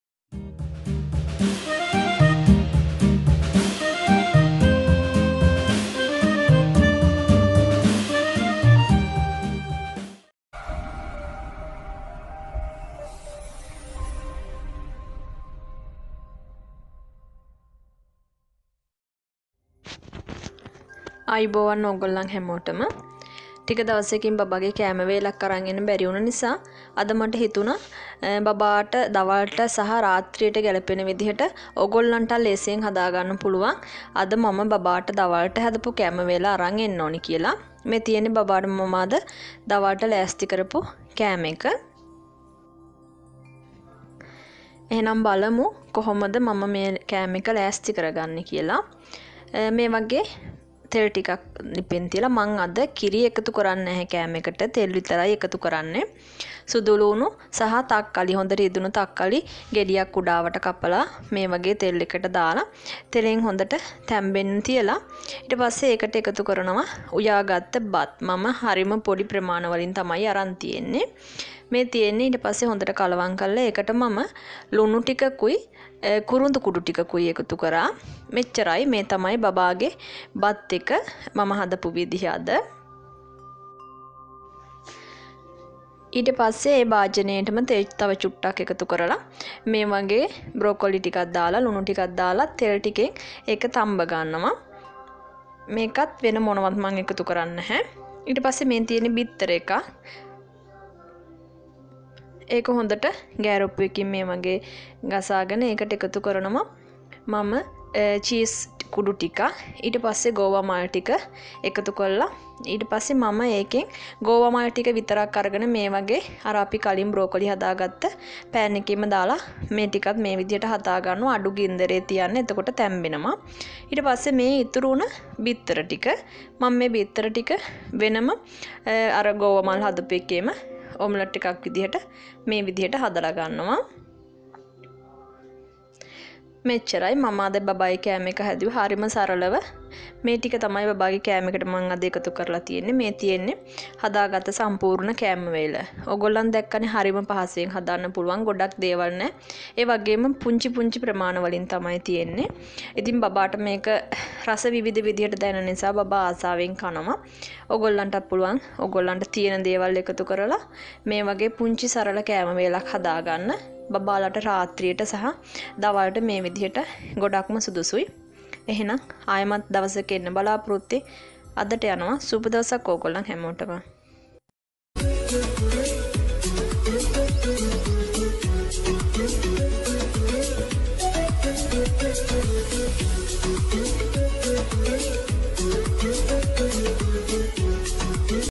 බබාලට දවල්ට සහ රාත්‍රියට ගැළපෙන්න කෑම වේලක් හදාගන්න හැටි / sinhala baby food recipe
හරිම සරලව තමයි මම මේ කෑම එක හැදුවෙ.. ගොඩක් එළවළු තම්බලා ඒවගෙ පෝශනය විනාශ කර ගන්නෙ නැතුව මම බබාලට මේ විදිහටයි කෑම හදන්නෙ .. විනාඩි කීපයකින් කෑම වේල ලෑස්ති කරගන්න පුළුවන් .

මෙතන තවත් රාත්‍රියට දවල්ට ගැලපෙන පුංචි ආහාර වට්ටොරු වල එකටුවක් තියෙනවා..

එකතු වෙන්න  මගේ instagram සහ facebook page එකත් එක්ක..